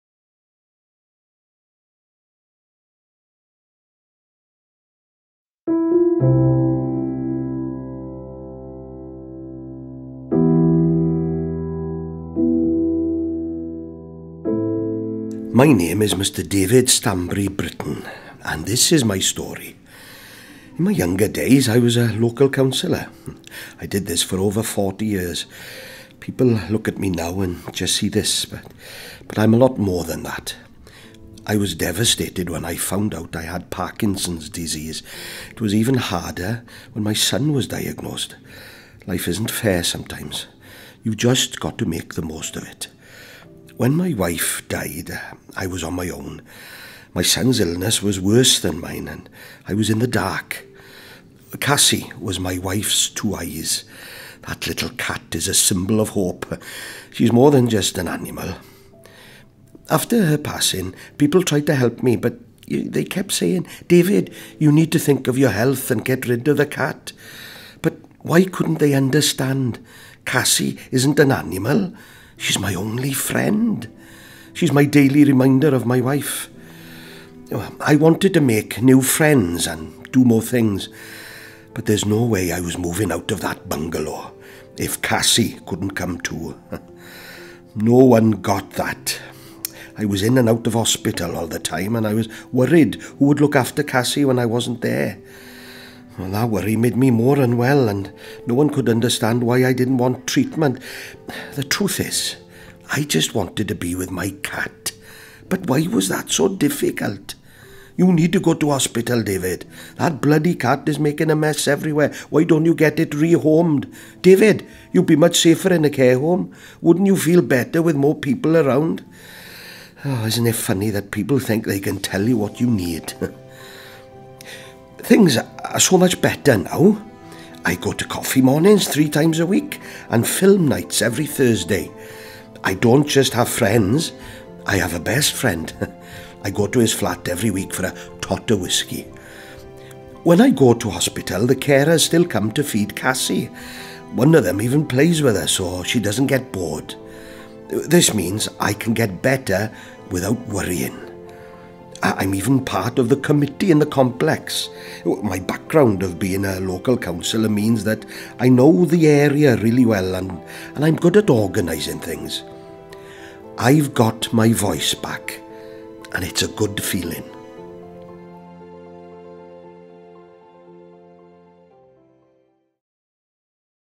What really mattered to me by Mr David Britton
This is Mr David Britton's story about the outcomes focused care he received from his social worker.